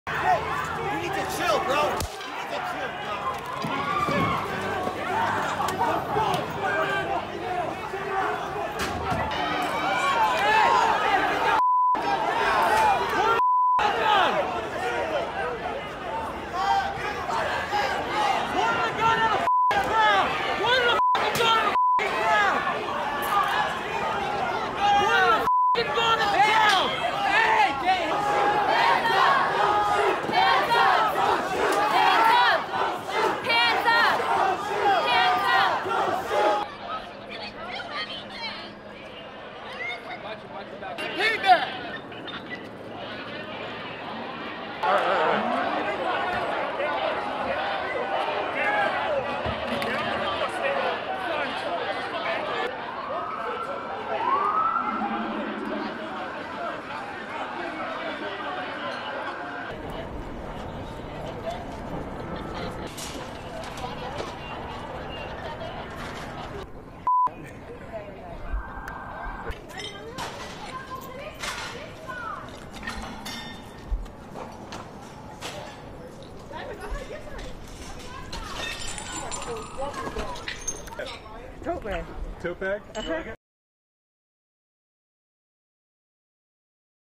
Police officer points gun at crowds in New York
An NYPD officer points his gun at protesters after a colleague was assaulted.

New York City Police Commissioner Dermot Shea says it would be one of six incidents to be investigated by the department's Internal Affairs Bureau.

A curfew failed to quell another night of violence in the city, with many shops - including Macy’s and Nike - hit by looters.

Mayor Bill de Blasio introduced an 11 pm to 5 am curfew, which will be moved to 8pm on Tuesday.

De Blasio and Governor Andrew Cuomo said the outbreaks of violence the previous two evenings gave them no choice but to impose a curfew, even as they insisted they stood with the throngs of peaceful demonstrators against police brutality and racial injustice.